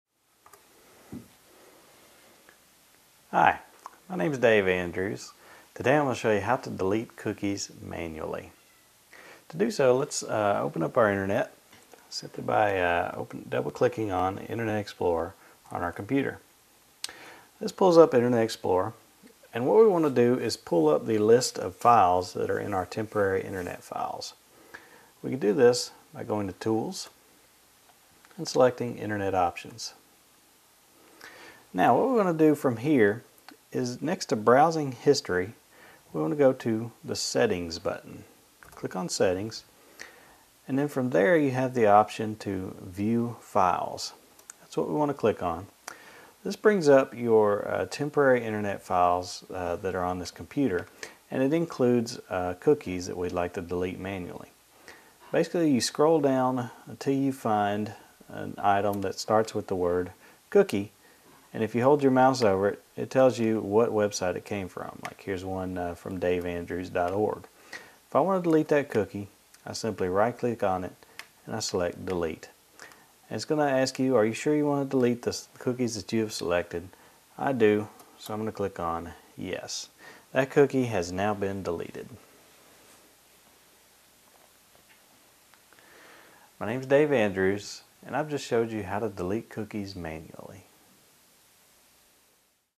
Computer & Internet Tips : How to Delete Cookies Manually
To delete cookies manually from an Internet browser, select the "Tools" menu, go to "Internet Options," click on the "Settings" button in the "Browsing History" menu and then look for files that start with the word "Cookie." Hold the mouse over cookie files to find out what Web site the cookie came from, and decide whether to delete the file manually with help from a software developer in this free video on deleting cookies.